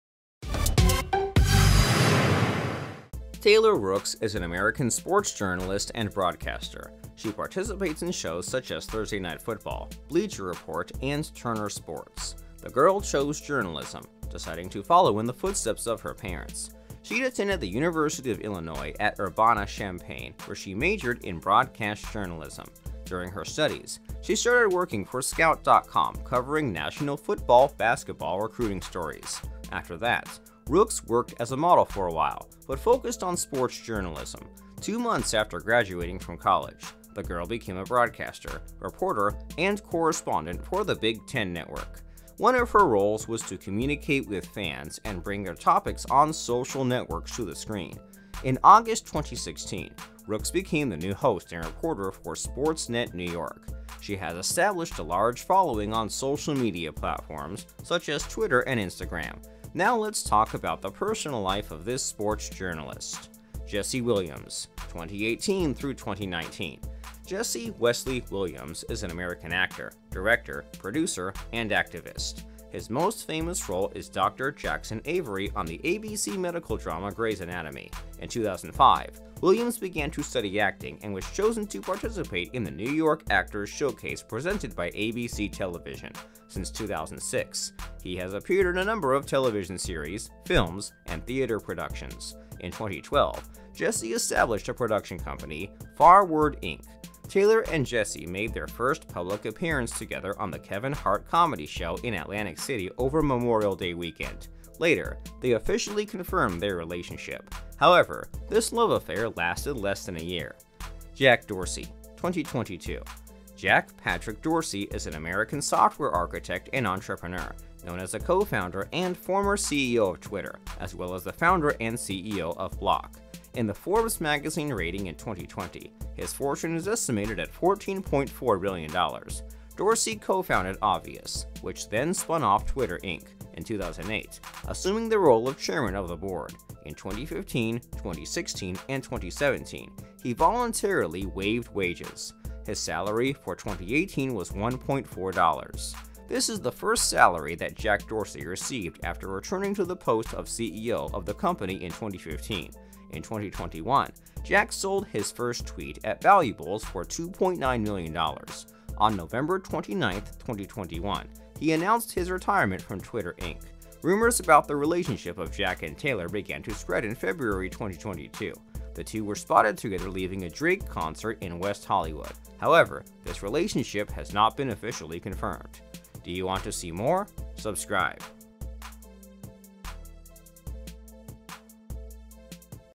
Taylor Rooks Boyfriends List (Dating History)
Taylor Rooks is an American sports journalist and broadcaster. She participates in shows such as Thursday Night Football, Bleacher Report and Turner Sports. The girl chose journalism, deciding to follow in the footsteps of her parents. Let's talk about the personal life of this sports journalist.

00:00 - Taylor Rooks Boyfriends
01:04 - Jesse Williams
01:52 - Jack Dorsey